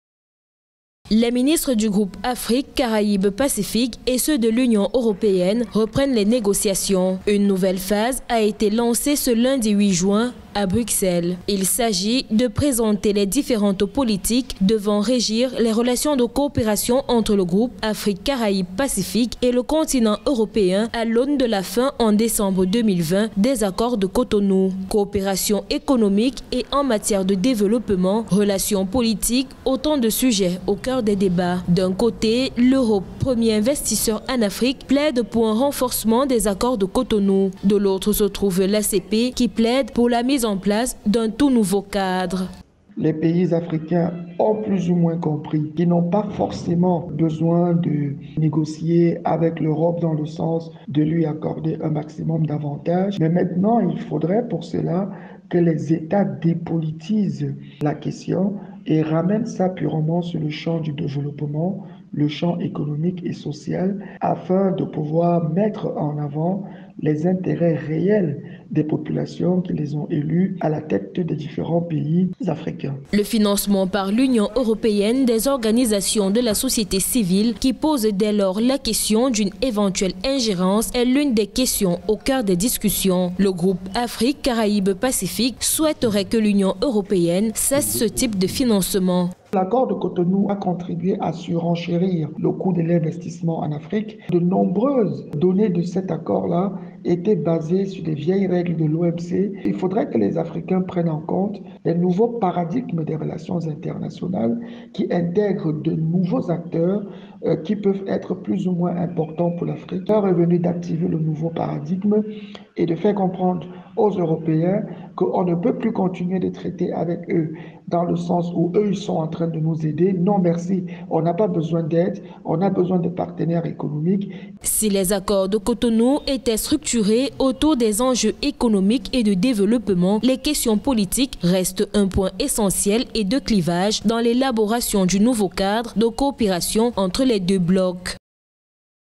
Afrique, VERS UN NOUVEL ACCORD ENTRE L’ACP ET L’UE
Politique : Les ministres du groupe Afrique Caraïbes Pacifique et ceux de l’Union européenne reprennent les négociations. Une nouvelle phase a été lancée ce lundi 8 juin à Bruxelles.

Catégorie